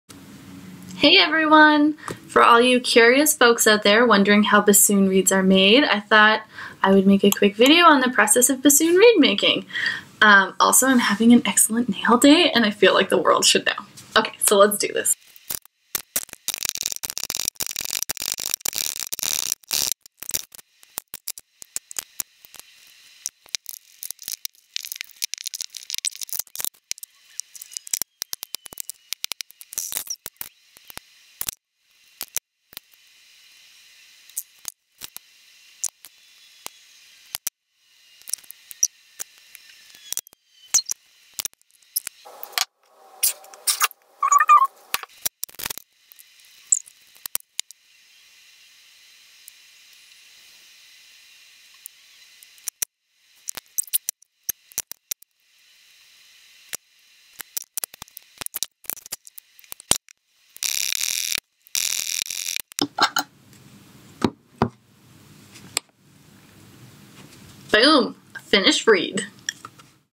Bassoon Reed Making in One Minute
Sped-up step-by-step video showing the process of how to make a bassoon reed - profiling through to tip finishing.

These steps include profiling, shaping, scoring, forming, wrapping, and tip finishing.